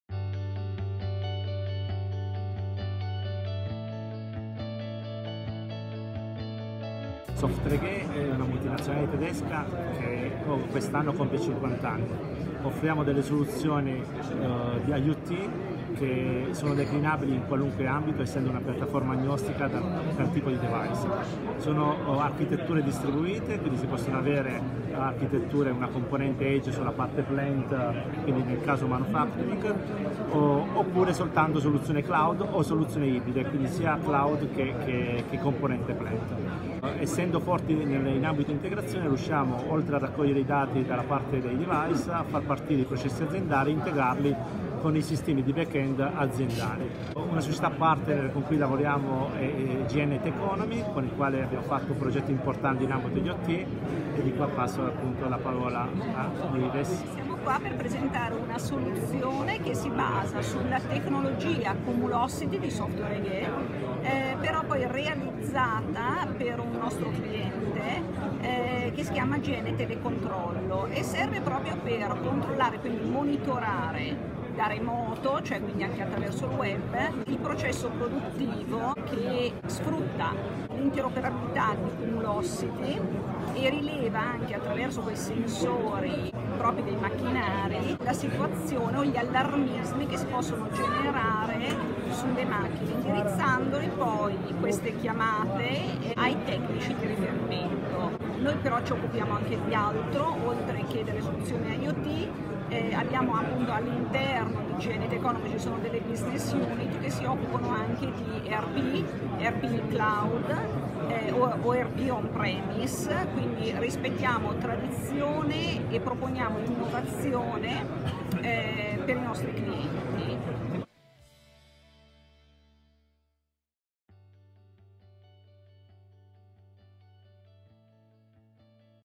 Forum Software Industriale 2019 - Intervista a GN Technomy e Software AG
Video intervista di Automazione Oggi a Nives Borroni, CEO di GN Technomy, e Francesco Borraccino, PreSales Manager Italy di Software AG, in occasione del Forum Software Industriale 2019